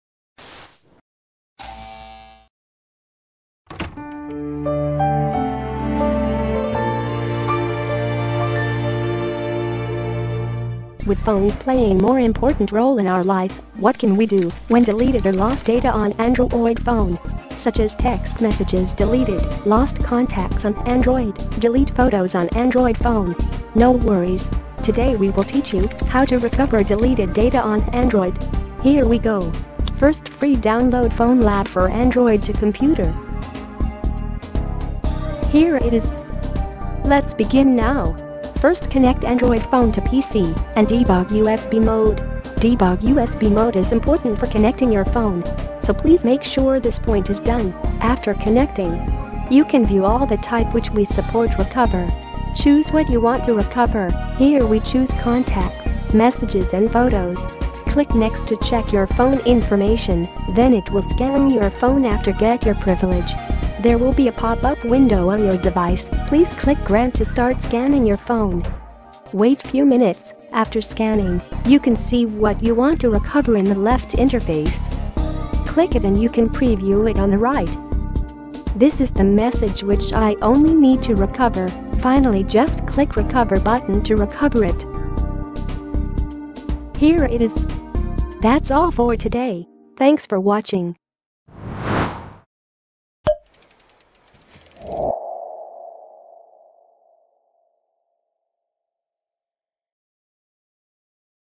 How to Recover Deleted Data on Android phone and tablet?
How to recover deleted files from Android phone?
Here is the video tutorial to show how to recover deleted or lost photos, videos, contacts, text messages, call log, WhatsApp, and more on Android phone.
Even if your Android phone is broken, password forgotten, or smashed without pressing to unlock it, you could also use Aiseesoft FoneLab for Android to recover the lost or deleted deleted data and more from Android phone.
Even without root, you could also extract the lost files from the password-forgotten phone.